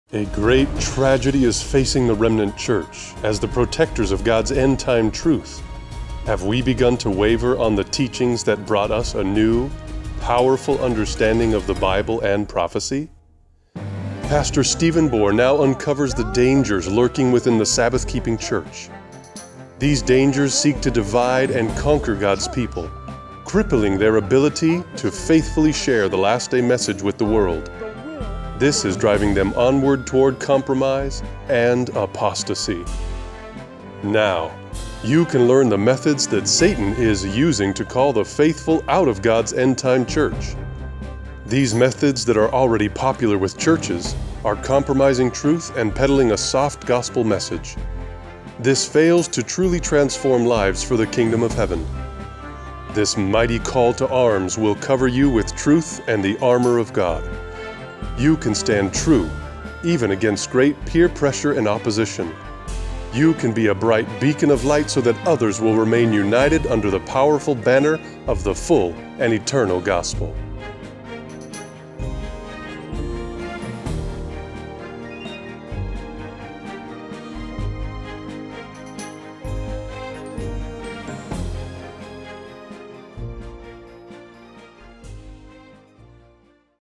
Worship at Satan's Throne Book Promo
Pastor Stephen Bohr has written a book entitled, "Worship at Satan's Throne."

By reading this book, you will learn the deceiving methods that Satan is now using to call the faithful out of God's end-time church.

This mighty call-to-arms will cover you with truth and the armor of God, so you can stand true even against great peer pressure and opposition and be a bright beacon of light so that others will remain united under the powerful banner of the full and eternal gospel.